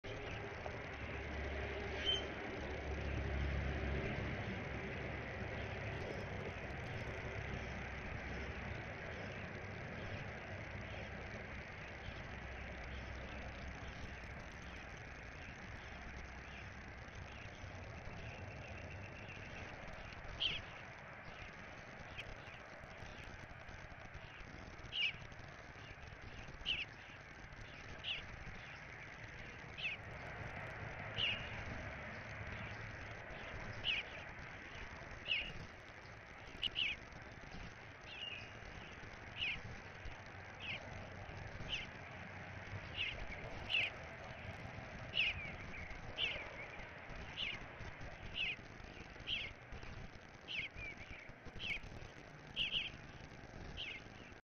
Eastern Bluebirds visit the pond
Recently, the Bluebird couple has started to bring babies to the backyard. They nested elsewhere for the first brood (I think the old female was driven out and the new one did not like my box at first), but they now have an active nest here with four eggs as of today. The babies still hang around, following the parents to feeder and bath. (I am livestreaming this cam on my website ostdrossel.com and on my channel on birdsy.com)